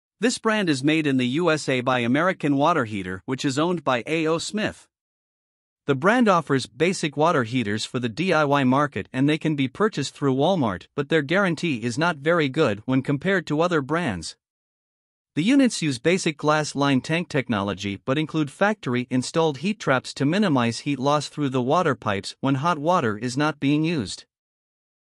HvacRepairGuy 2023 US Craftmaster Electric Storage Water Heater Reviews
In this video, HvacRepairGuy reviews the 2023 US Craftmaster brand electric storage water heaters.

You can get the reviews of all of the brands